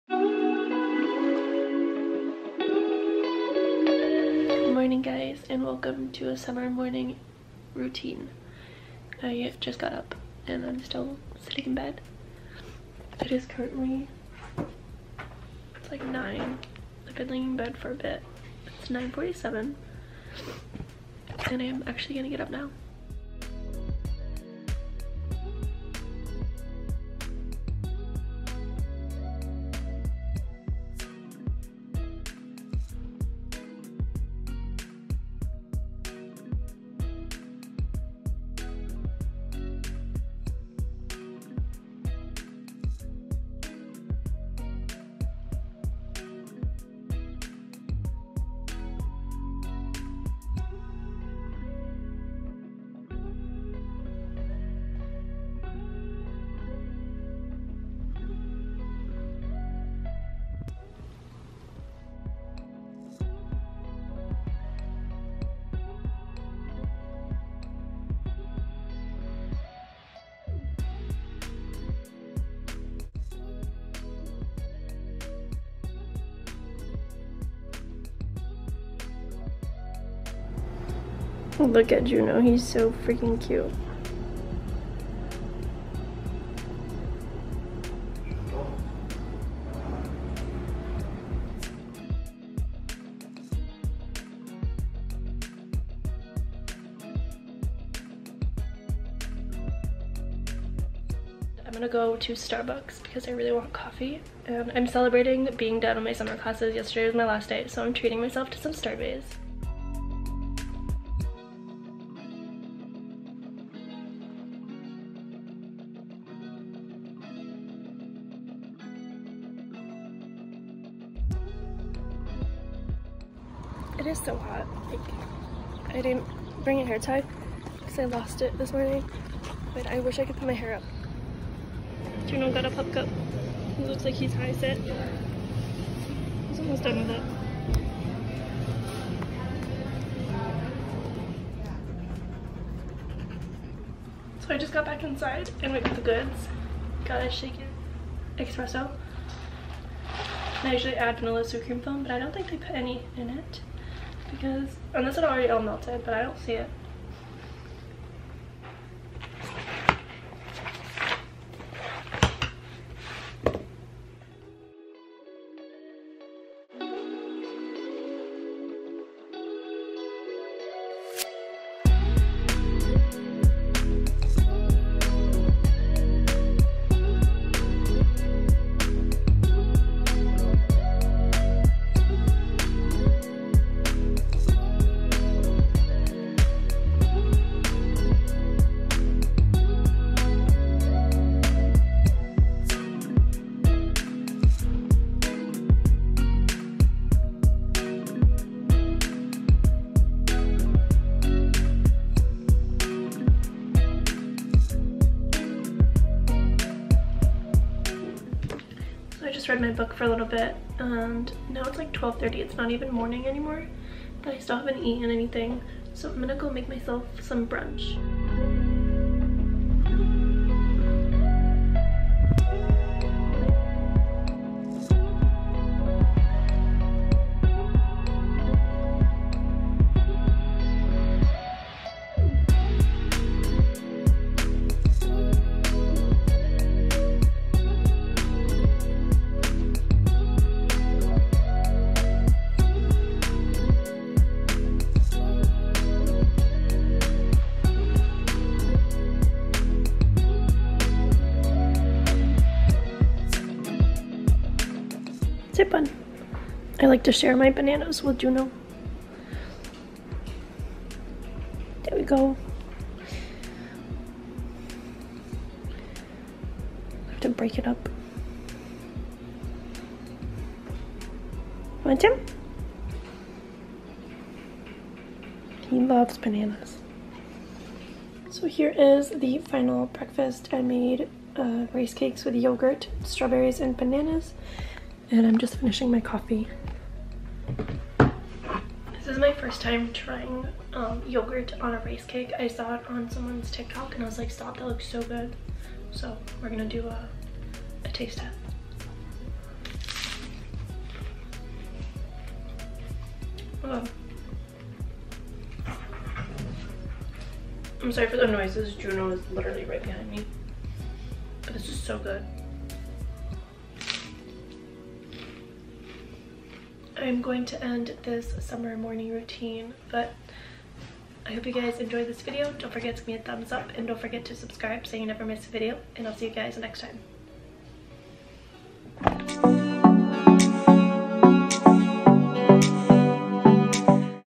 *realistic* summer morning routine
here is my very realistic summer morning routine! It was a realistic morning during summer & a pretty chill since I just finished my summer classes. I slept in, went for a walk, got starbucks, read my book and made breakfast.